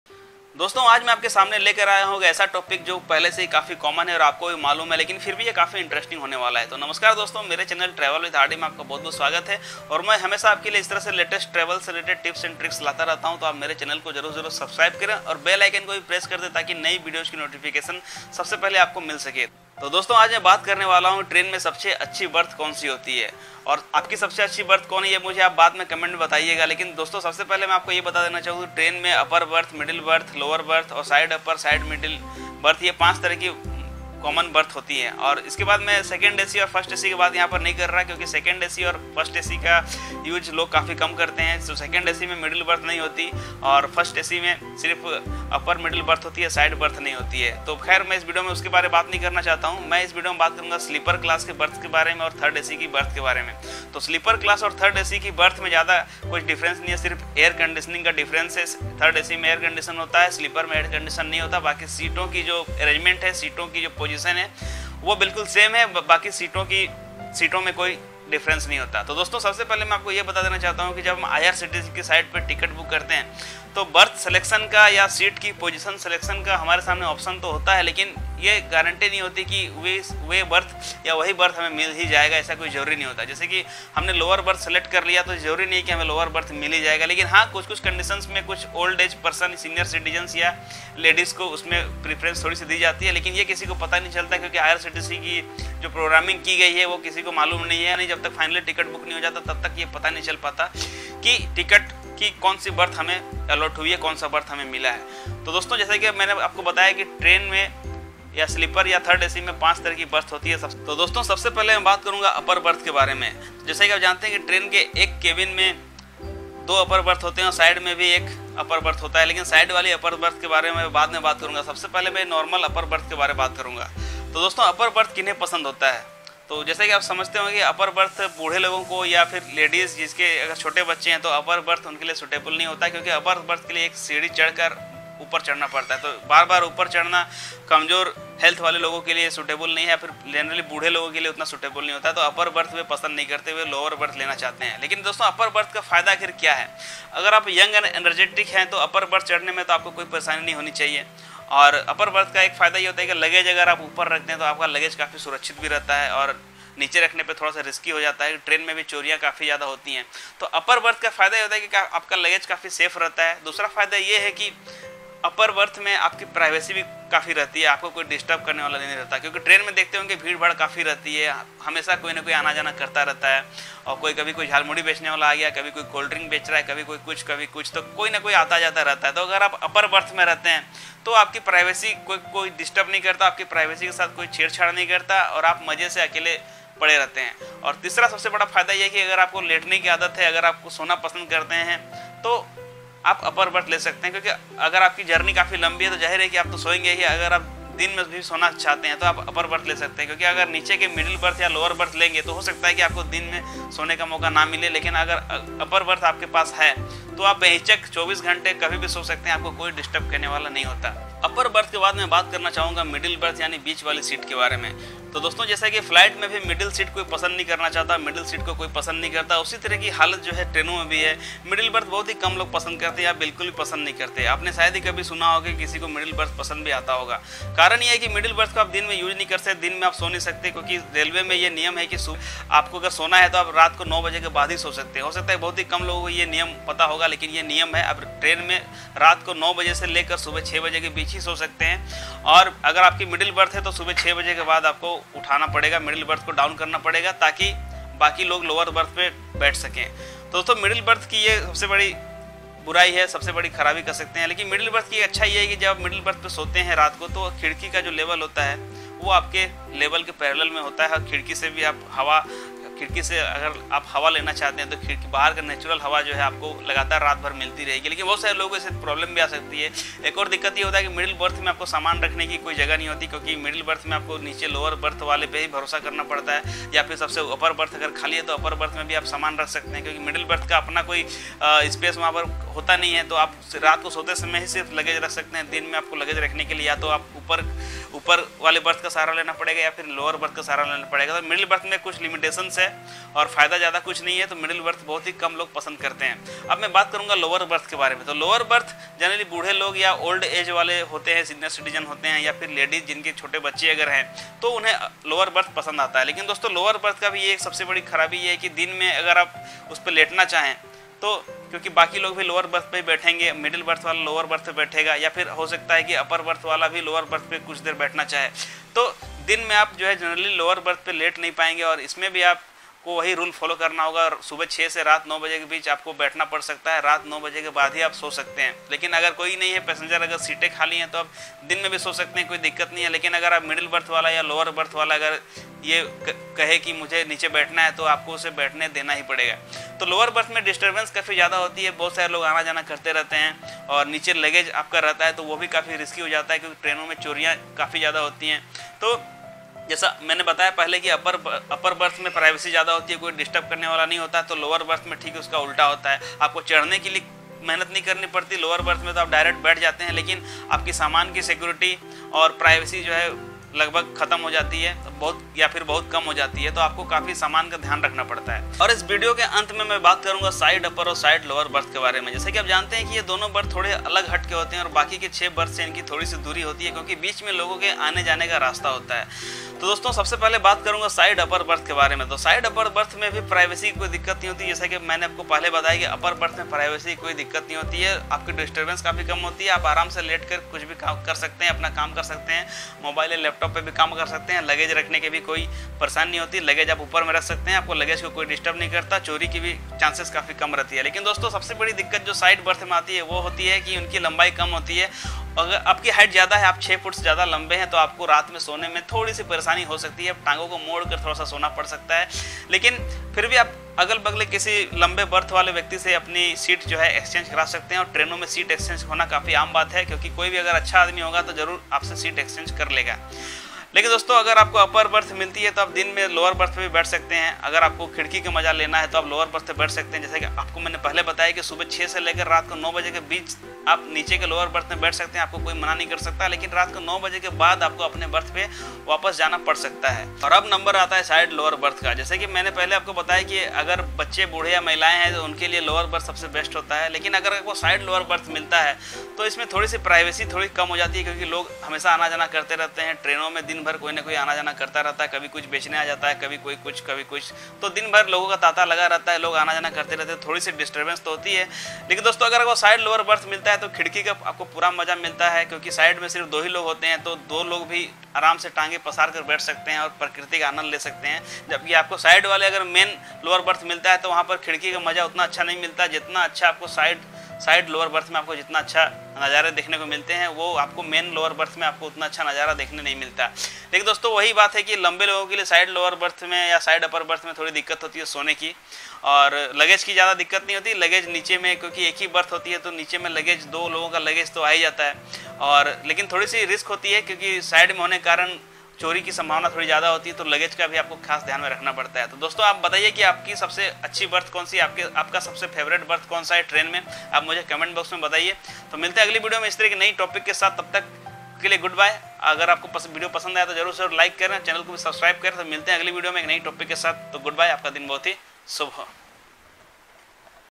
Which is the best berth in trains?
इस विडियो में मैं बात करूँगा की ट्रेन में सबसे अच्छी बर्थ कौन सी होती है?
I will discuss about which is the best berths in Indian trains among upper berth, middle berth, lower berth, side upper berth and side lower berth.